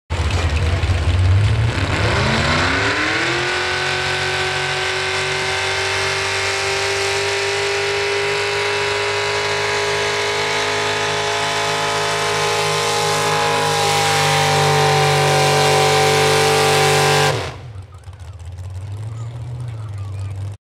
Daren Moore-NK Pullers Adams,Ne 2016
Daren pulling "Red Neck Express" in the 2Wd modified class